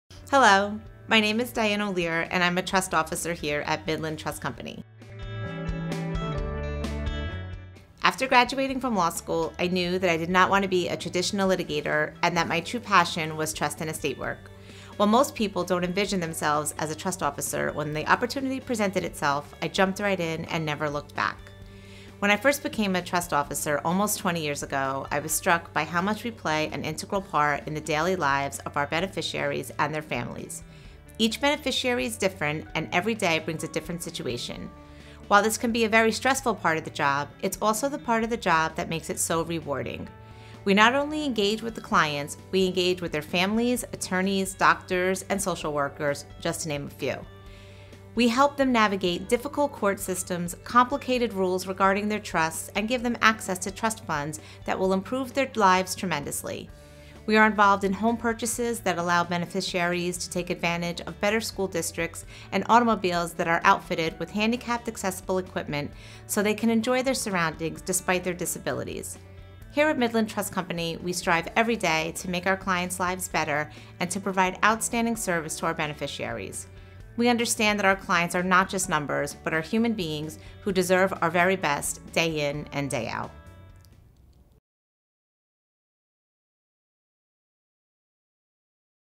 Diane Olear, JD – Senior Trust Officer – Midland Trust Company
Senior Trust Officer, Diane Olear, JD, shares a personal introduction, including background and experience. Disclosures *Promoted since video recording.